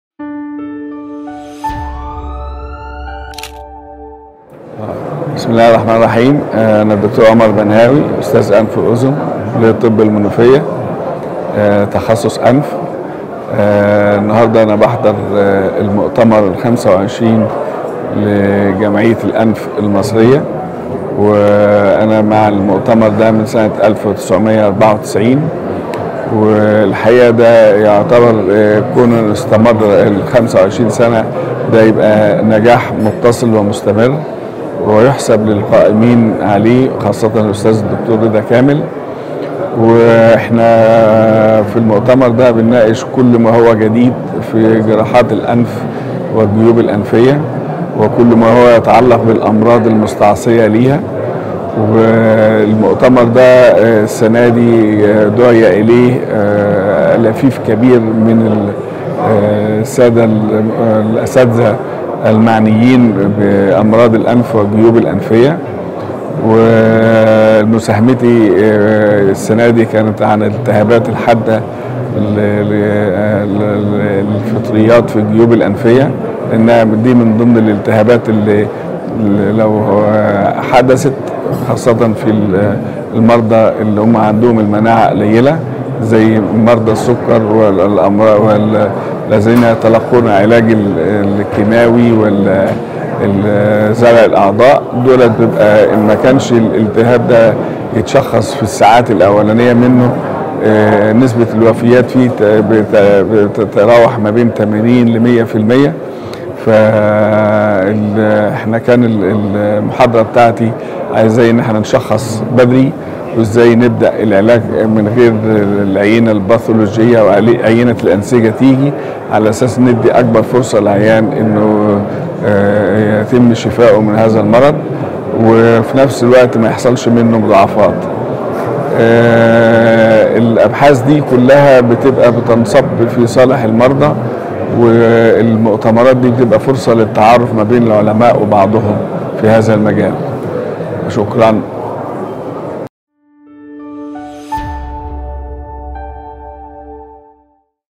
Prof Omar Banhawi talks about the conference and his talk
Prof  Omar Banhawi
Professor of ENT, Faculty of Medicine, Menoufia University